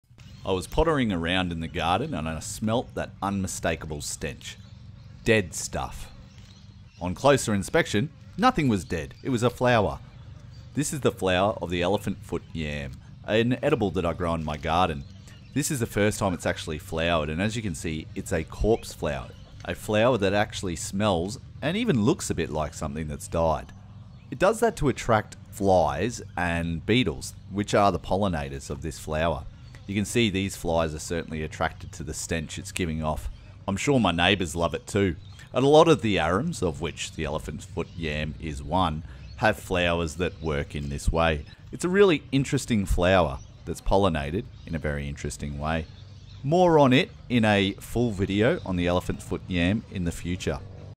Corpse Flower of the Elephant Foot Yam
In this video I discuss the rotten smelling corpse flower of the Elephant foot yam.